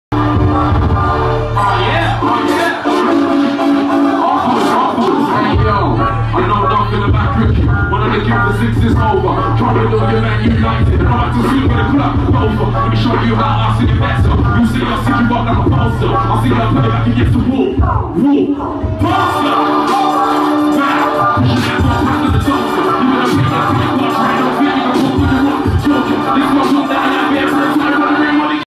P Money goes in at Eat Your Greens
Eat Your Greens October 2014
The Kazimier - Liverpool

P Money
Uncle Dugs
The Busy Twist